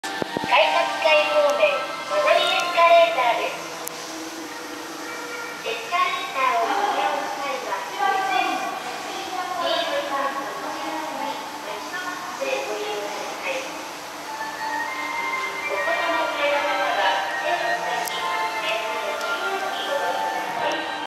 北海道エスカレーター動画（札幌市）4K対応その169 Videos taken in Hokkaido
札幌市白石区にある南郷７丁目駅のエスカレーター（フジテック製）の動画その２です。２０２０年４月３日撮影。
Escalator in Sapporo City part 169